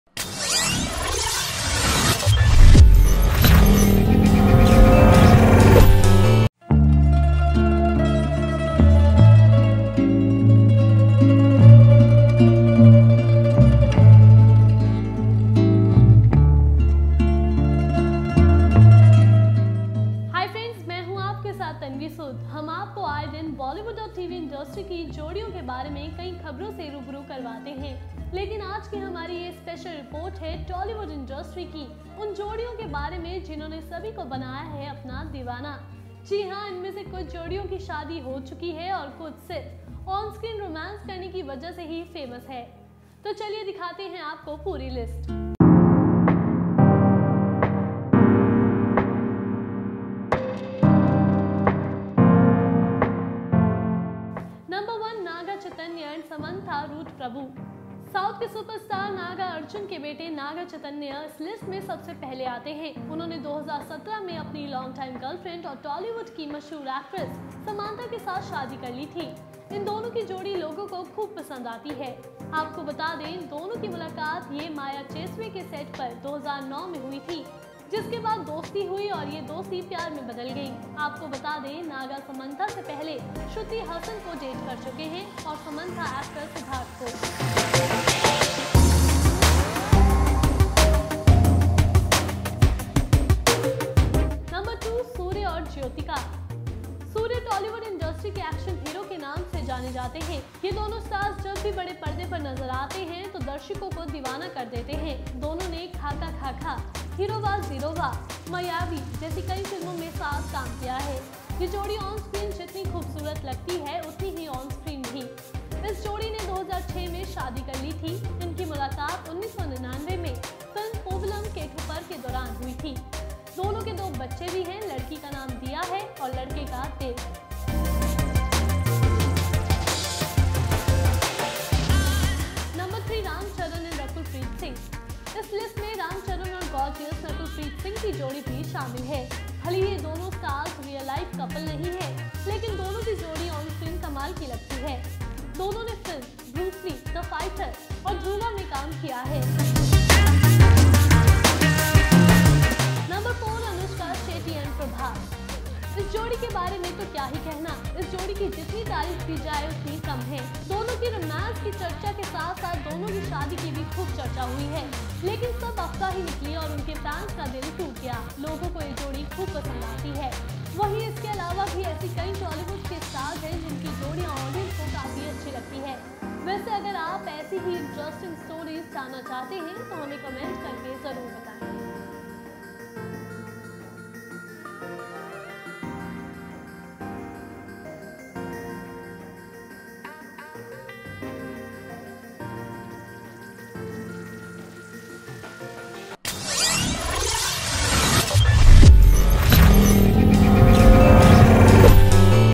ये है Tollywood की वो Blockbuster जोड़िया, जिनके आगे है सब पानी कम चाय
हम आपको आए दिन bollywood और tv industry की जोड़ियों के बारे में कई खबरों से रूबरू करवाते है.. लेकिन आज की हमारी ये स्पेशल रिपोर्ट है tollywood industry की उन जोड़ियों के बारे में.. जिन्होनें सभी को बनाया है अपना दीवाना …